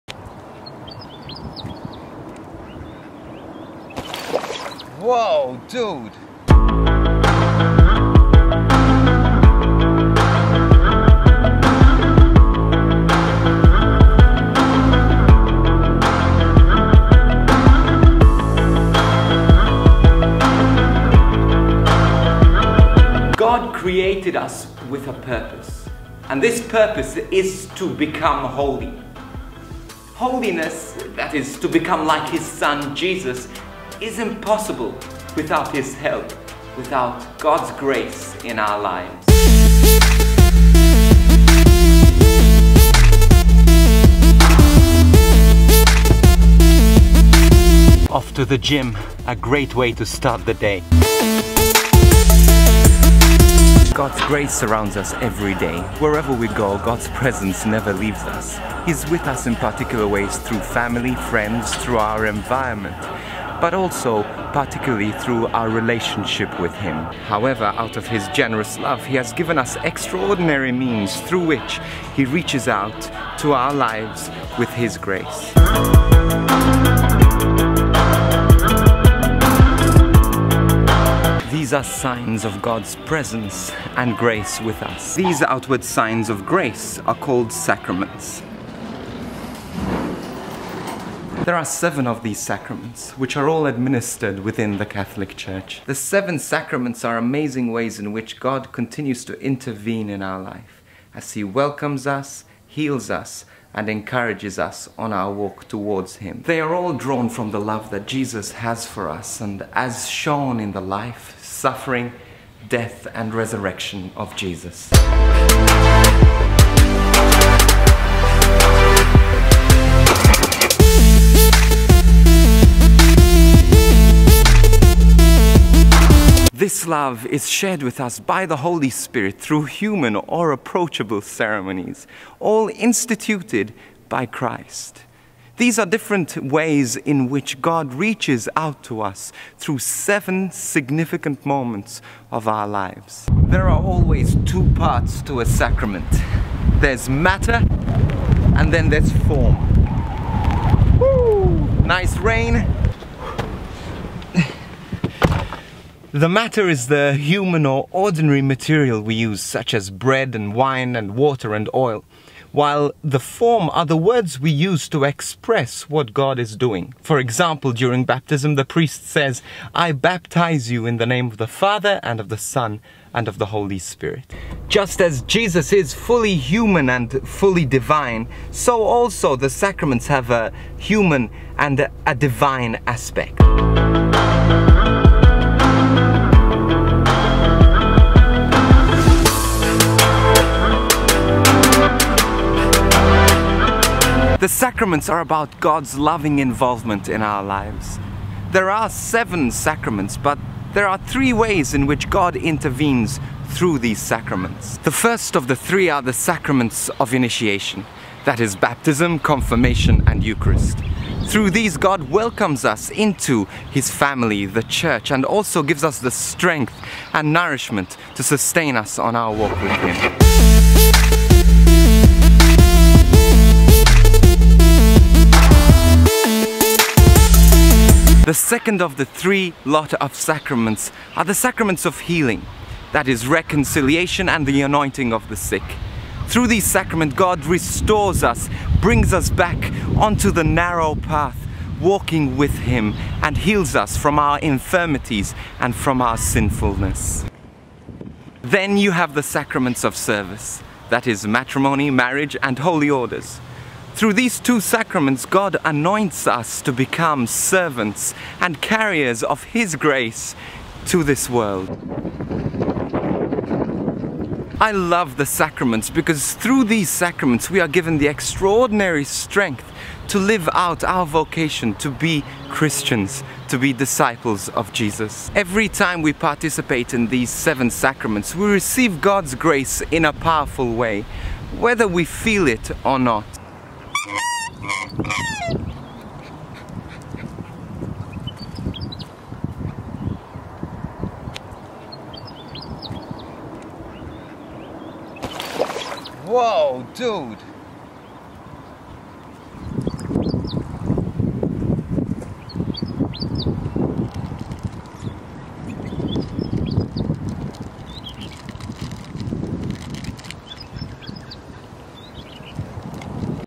The Sacraments (An Introduction) - Fr. Rob Galea
Catholic Speaker, Fr Rob Galea, gives an overview of the Sacraments of the Catholic Church and how we can live out the grace given to us through these sacraments.
Fr Rob talks about the purpose of every human being, to become holy like Jesus and how the sacraments help us to be holy.
Fr Rob discusses the purpose of the sacraments and explains the difference between the Matter and Form involved in receiving a sacrament.
Filmed in Bendigo, Victoria, Australia
Catholic Teaching Resources for to accompany this video is available
Encounter Teaching Resources for the Sacrament s of the Catholic Church
7. Anointing of the sick
Further Reading:
FRG Ministry
Strathdale,
Victoria, 3550